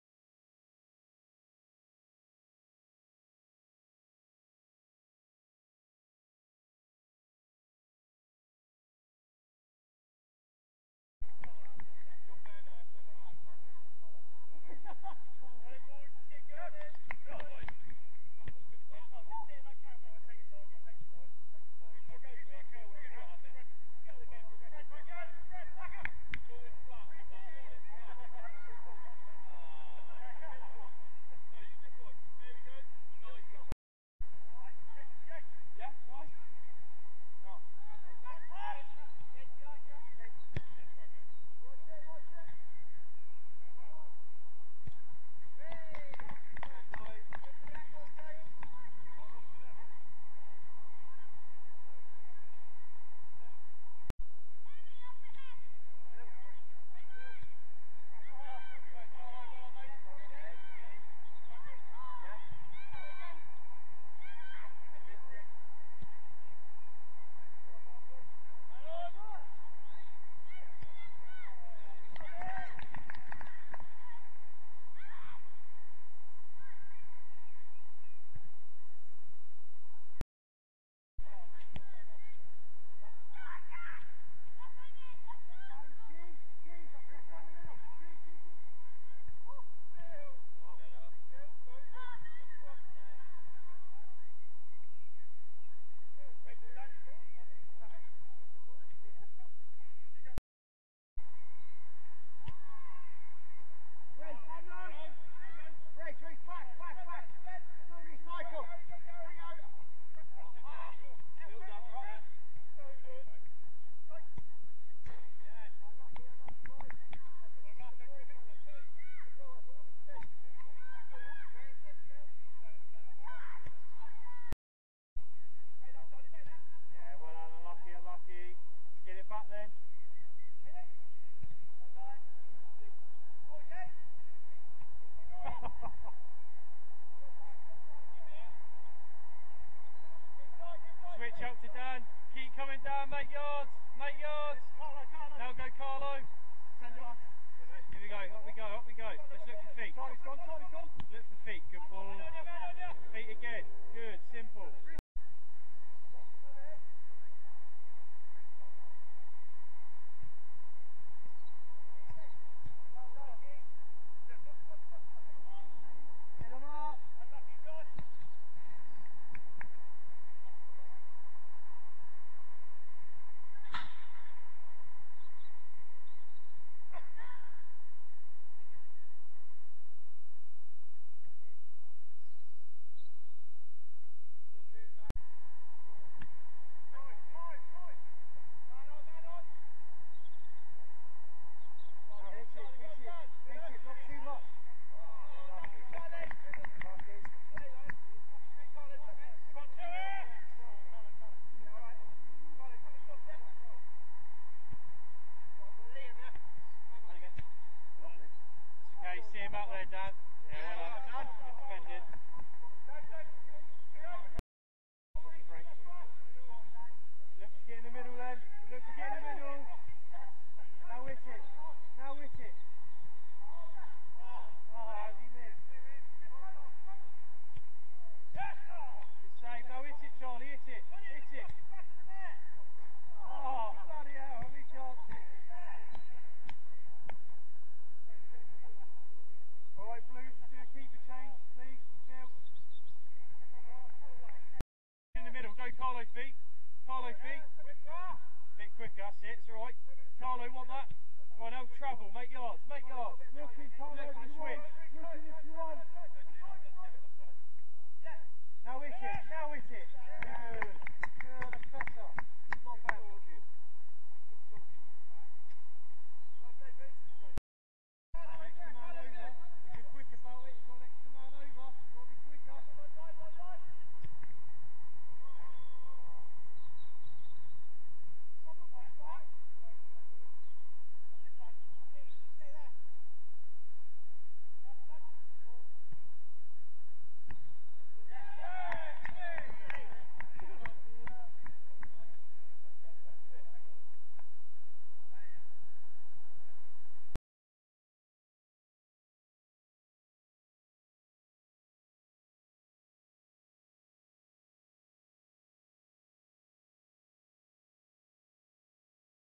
Friday Night Football @the County Ground 30th April 2021
Friday Night Football is where it all started for Academy FC, a bunch of lads meeting up and playing 60 minutes of football on a Friday nothing better.